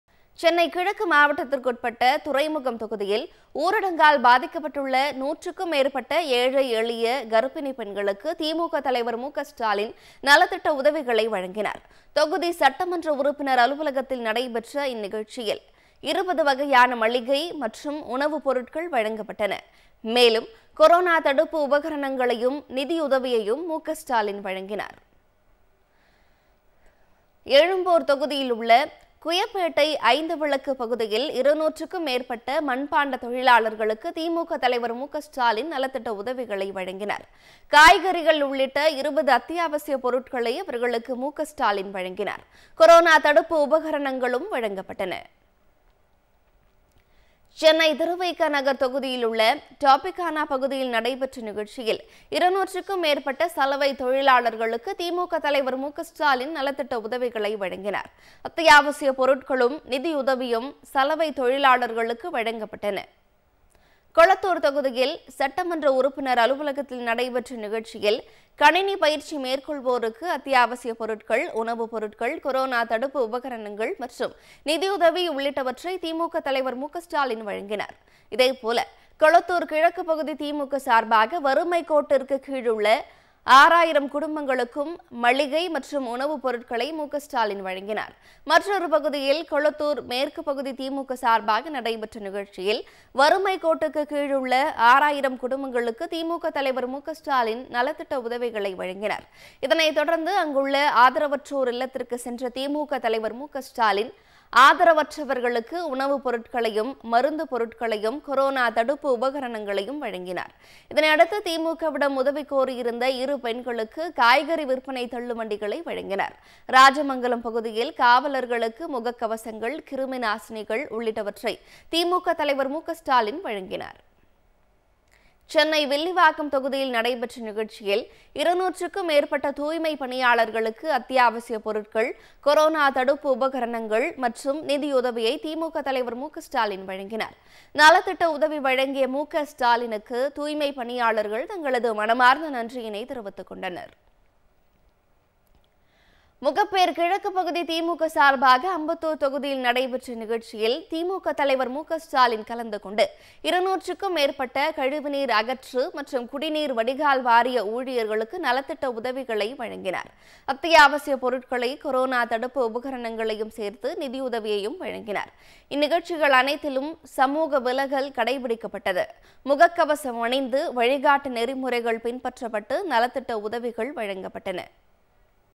இன்று (19.04.2020) சென்னையில் உள்ள ஆறு தொகுதிகளில் மக்களுக்கு பல்வேறு நலத்திட்ட உதவிகளை வழங்கினேன்.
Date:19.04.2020